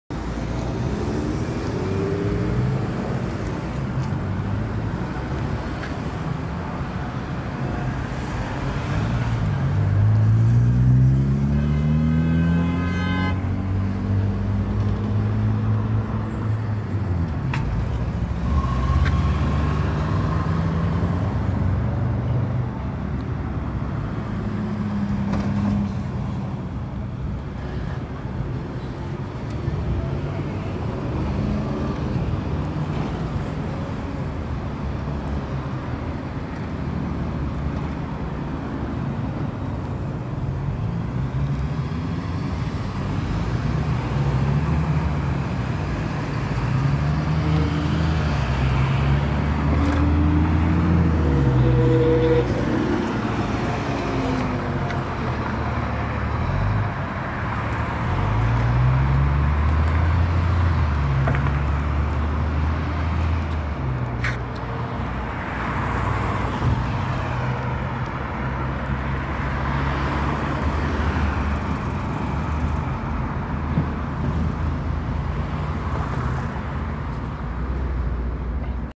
SEPTA's 2005 New Flyer D40LF #8026 on 12 to 49th-Woodland
This is the same bus that I recorded in a previous video going to Columbus-Dock